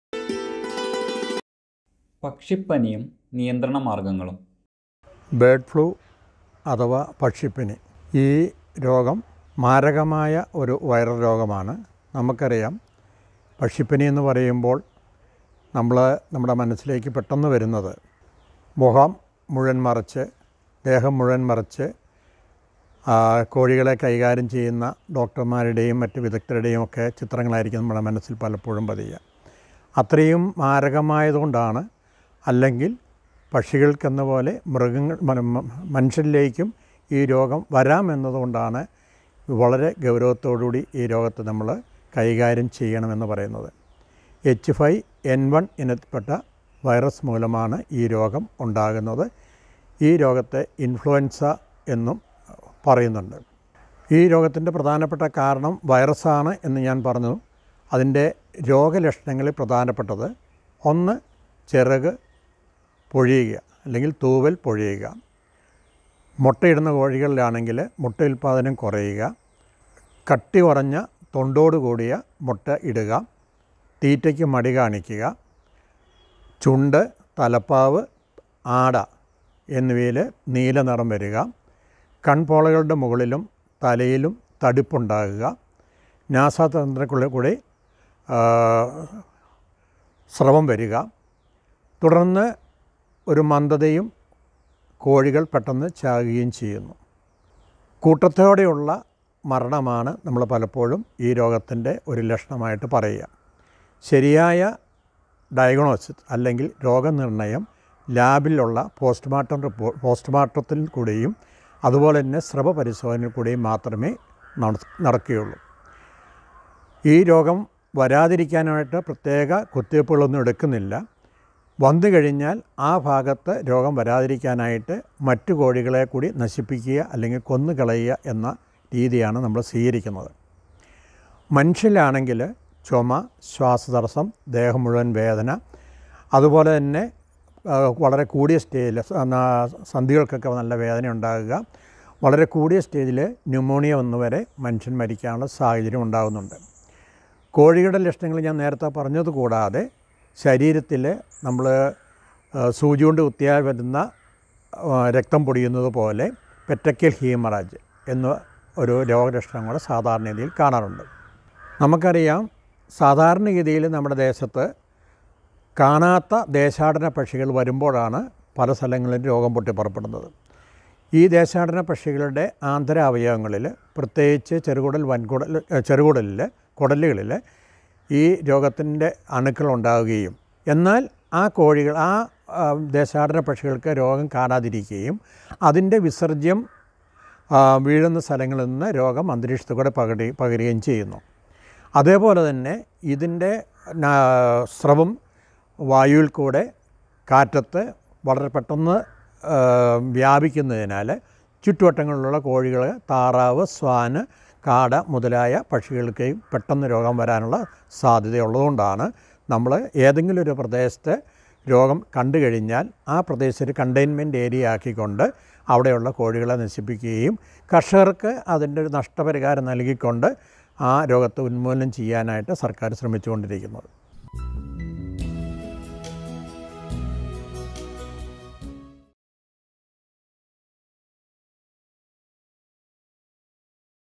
പക്ഷിപ്പനിയും നിയന്ത്രണ മാര്‍ഗങ്ങളും | Bird Flu Disease Management in Poultry
മാരകമായ രോഗം ആണ് പക്ഷിപ്പനി, ഇത് മനുഷ്യരിലേക്കും പകരാം. H5N1 ഇനം വയറസ് മൂലം ആണ് ഇത് ഉണ്ടാകുന്നത്. തൂവല്‍ പൊഴിയുക, മുട്ട ഉല്‍പാതനം കുറയുക, മുട്ടയുടെ തോടിന് കട്ടി കുറയുക, ചുണ്ടിലും താടയിലും തലപ്പാവിലും നീല നിറം വരിക, കണ്‍പോളകളുടെ മുകളില്‍ തടിപ്പ് ഉണ്ടാകുക എന്നിവയാണ് പ്രധാന രോഗ ലക്ഷണങ്ങള്‍. രോഗം പടരാതെ ഇരിക്കാന്‍ രോഗം വന്ന പക്ഷികളുടെ കൂടെ ഉള്ള പക്ഷികളെ ഉള്‍പ്പെടെ നശിപ്പിക്കുക എന്നതാണു പൊതുവേ ചെയ്യുന്നത്. രോഗം പടരാതെ ശ്രദിക്കുന്നത് ആണ് പ്രധാനം.